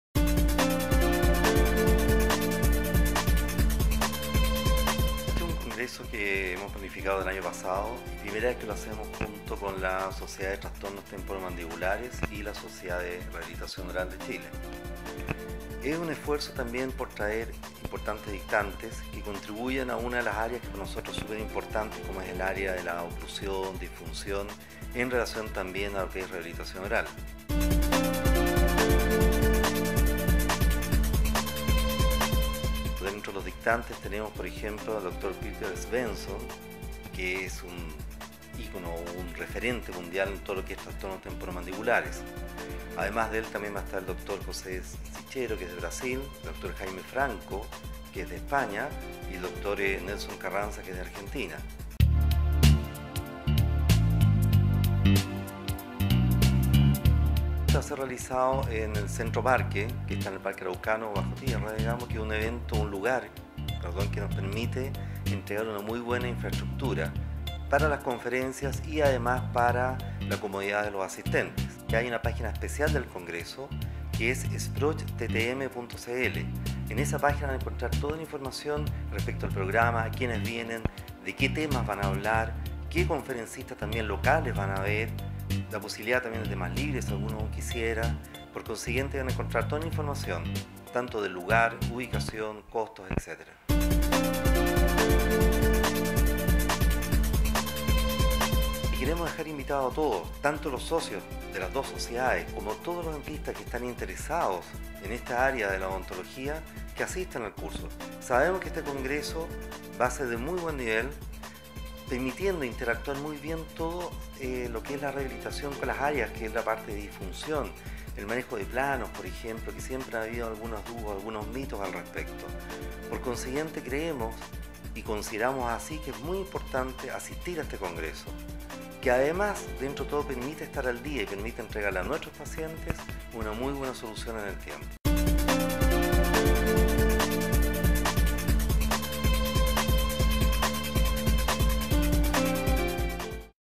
1er Congreso Internacional SPROCH TTM (Chile)
Dr. Héctor González nos habla acerca del 1er Congreso Internacional de Rehabilitación Oral  - Trastornos Temporo Mandibulares y Dolor Orofacial (21, 22 y 23 de agosto, Santiago)